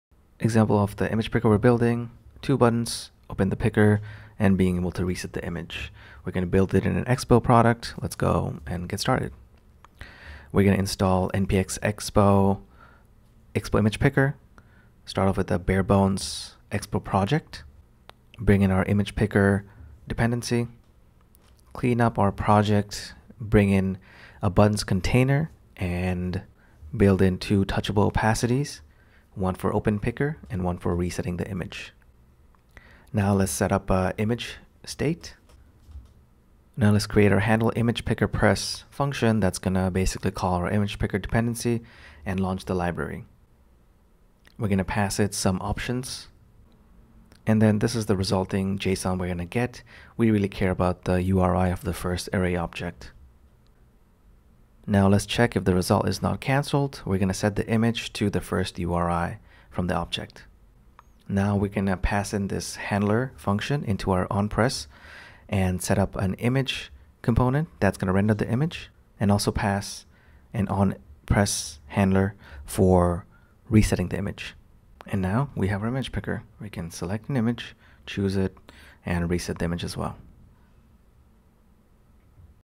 React Native Expo Image Picker
Hey Everyone!

A quick video on how to use the expo image picker!
Happy coding